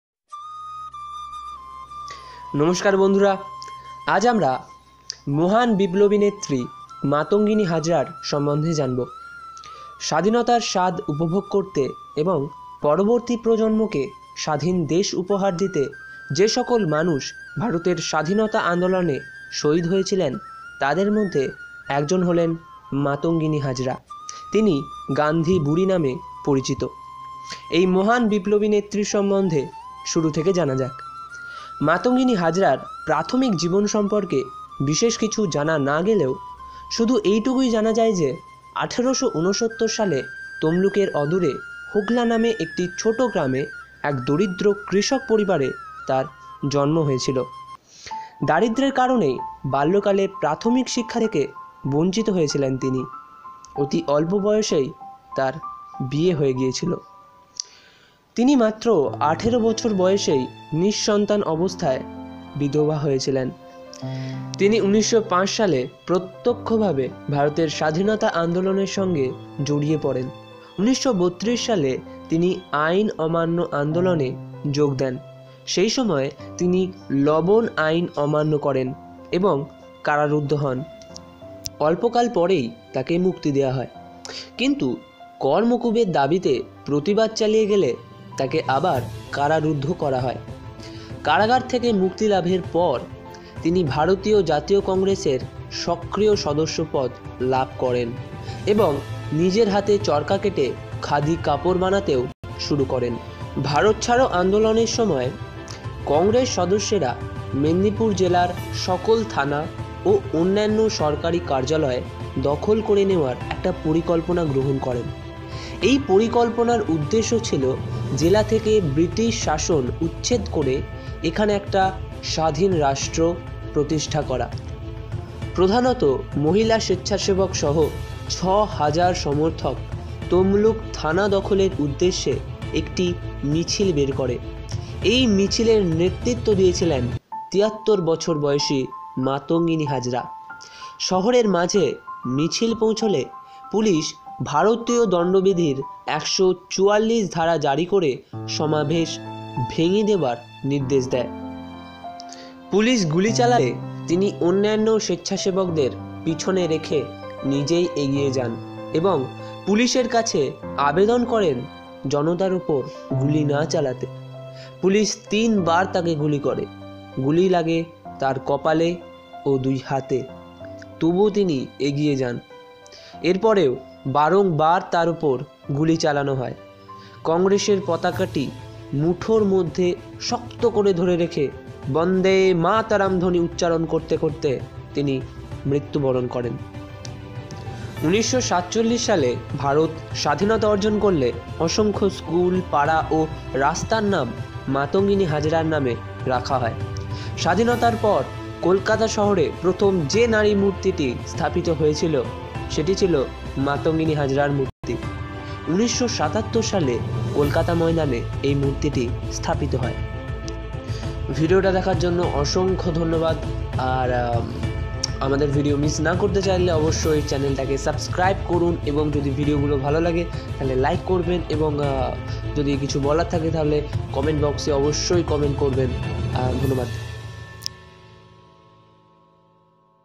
Matangini Hazra | Biography in Bengali
|| এক মহান বিপ্লবী নেত্রীর জীবন কাহিনী ||

  এই video টির বিষয় হল মাতঙ্গিনী হাজরার জীবনী/ biography in Bengali ৷৷ মাত্র ১৮ বছর বয়সে নিজের স্বামীকে হারিয়ে,তিনি ভারতের স্বাধীনতা আন্দোলনে জড়িয়ে পড়েন৷১৯৪২ সালে ভারত ছড়ো আন্দোলনের নেতৃত্ব দিতে গিয়ে,পুলিশের গুলিতে তাঁর মৃত্যু হয়৷৷

 More videos by Ikeen Bengali